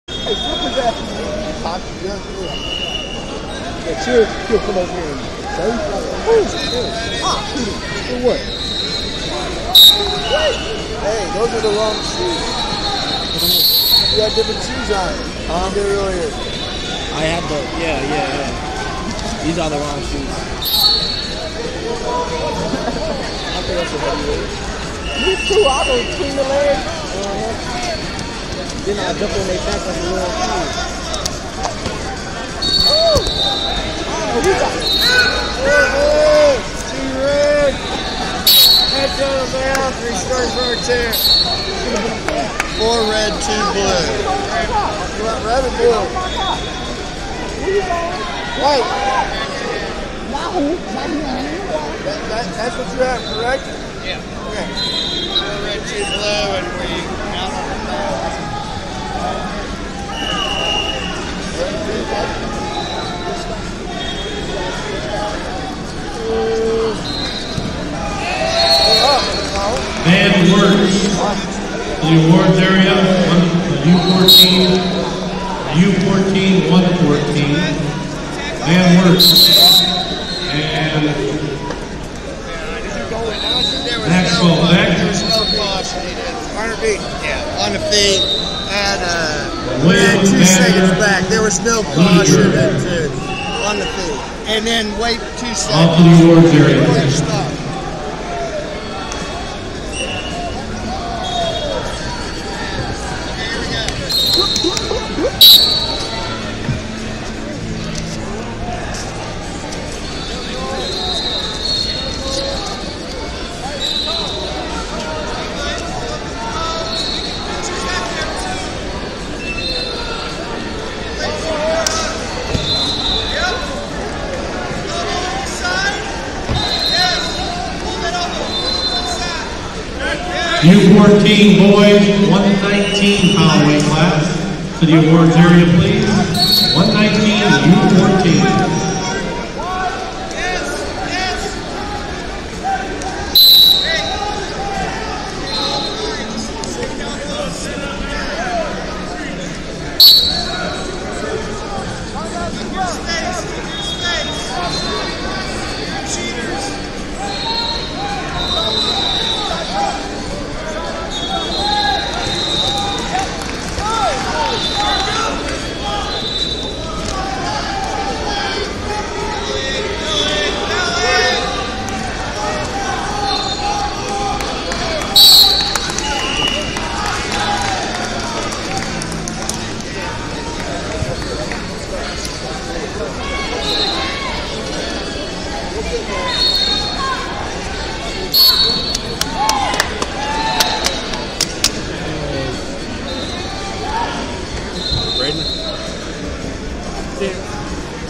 285 Lbs Cons. Round 1 - Trey White, IN Vs Bruce Wagers, OH 48b0
2022 Central Regional Championships - 285 Lbs Cons. Round 1 - Trey White, IN Vs Bruce Wagers, OH 48b0